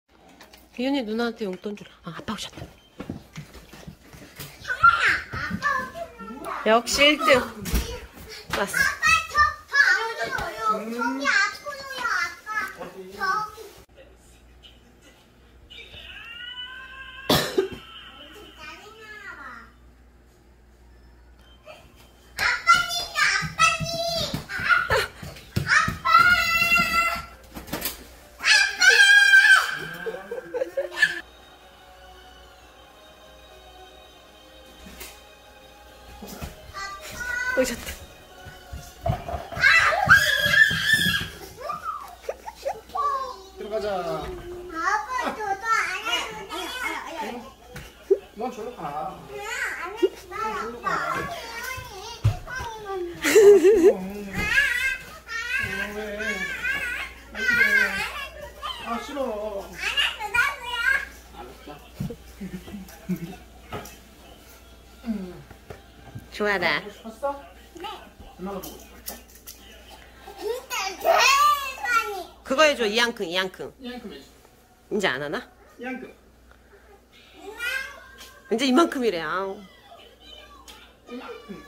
[일상]송송이네 집에 아빠가 오시면?(필로티2층😂아빠는낮에 옵니다😄)
매일매일 아빠가 들어오시는 도어락소리가 들리면 송송이들 셋이 출발!
나가실때 들어오실때 항상 같은 루틴!
이맛에 아빠는 오늘도 열심히 일을합니다ㅎㅎ

송송이네 일상사진과 이야기 구경오세요!
인스타그램 "송송이네"